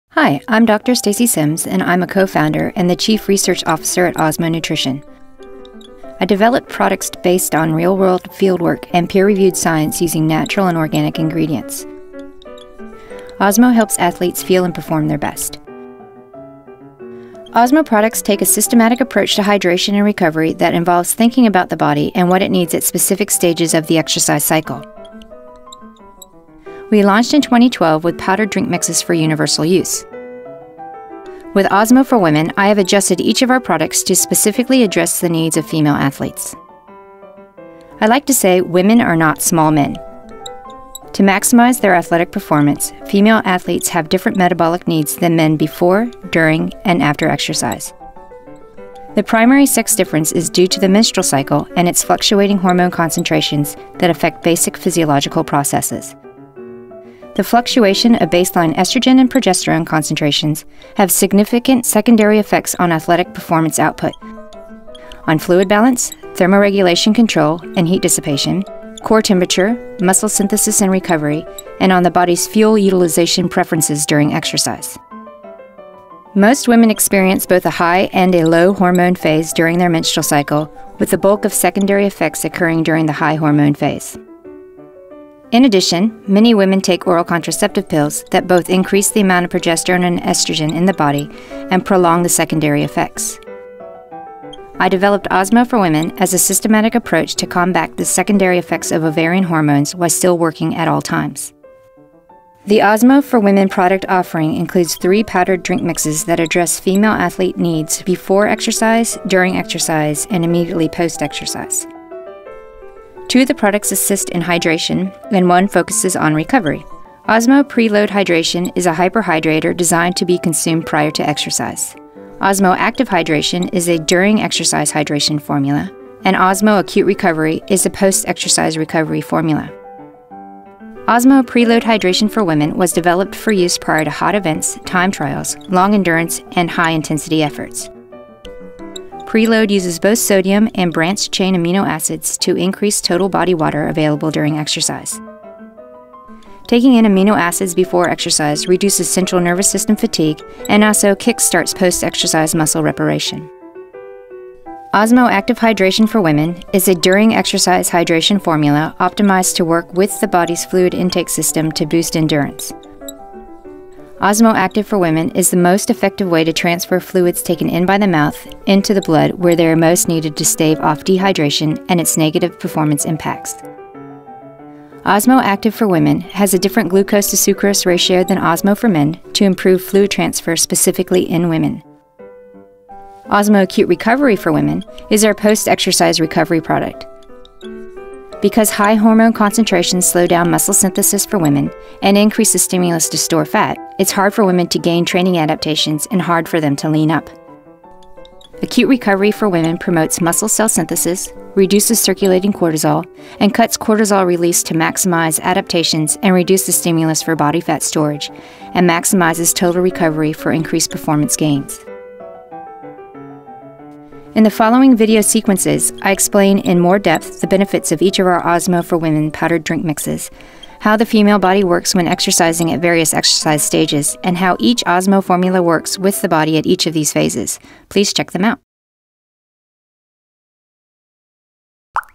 Osmo Nutrition for Women Overview
Osmo co-founder and Chief Research Officer, Dr. Stacy Sims explains the science behind the development of Osmo Nutrition's Hydration and Recovery products for women.
Video directed by GroundLevelResearch.com